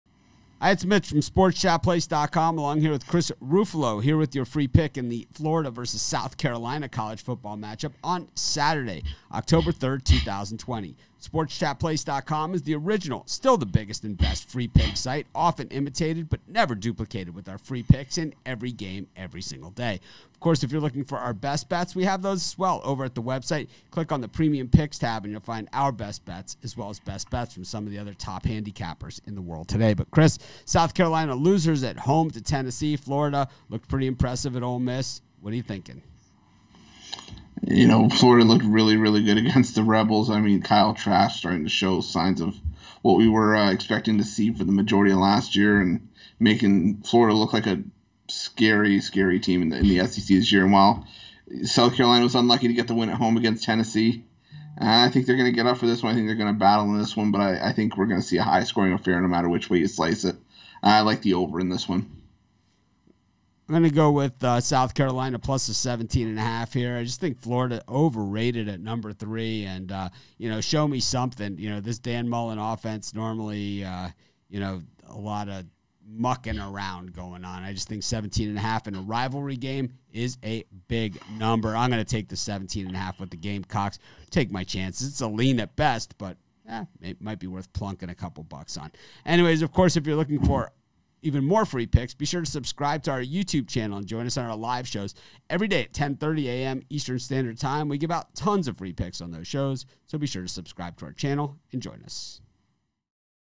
South Carolina at Florida College Football Picks & Prediction Saturday 10/3/20 | Sports Chat Place
South Carolina at Florida College Football Picks & Prediction Saturday 10/3/20

College Football Week 5 for the 2020 Season is here and we have all your betting needs covered. Sports Chat Place experts Mitch Wilson and Chris Ruffolo have the Free Betting Pick and Prediction in this South Carolina at Florida matchup for Saturday October 3, 2020.

Get the hottest sports betting picks, odds, and predictions daily. It's about all the Sports Chat Place as we put the money in your pocket.